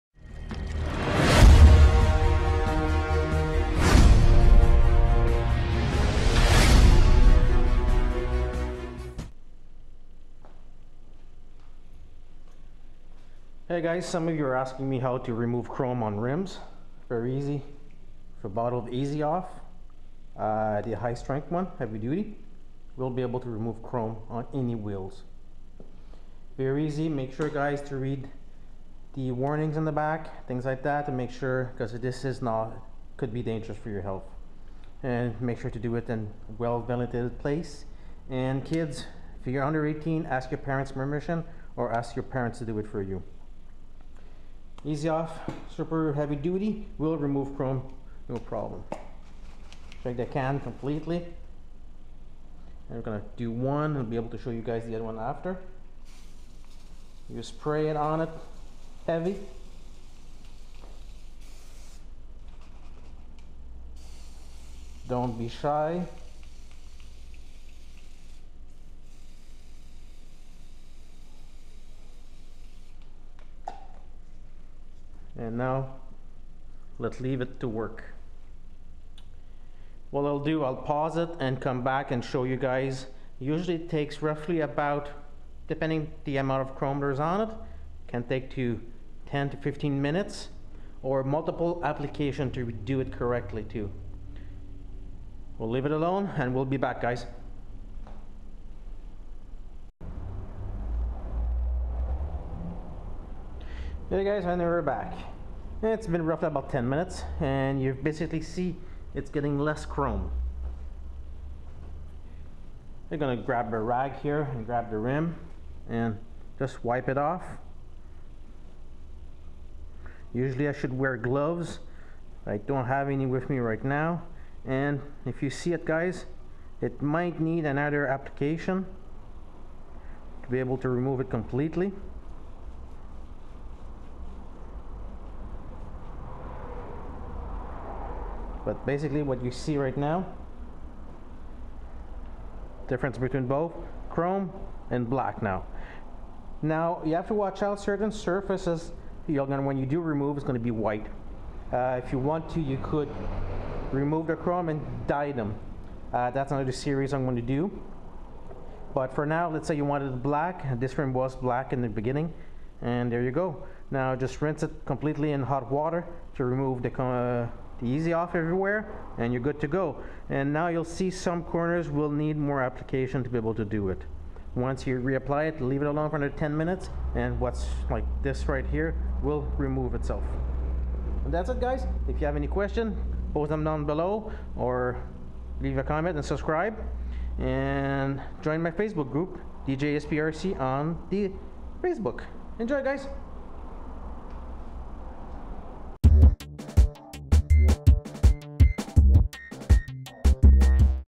HOW TO REMOVE CHROME ( R/C )
HOW TO REMOVE CHROME ON R/C WHEELS

WITH EASY-OFF   IN A YELLOW CAN

JOIN ME ON FACEBOOK  ( DJSPRC )

TWITTER  ( DJSPRC )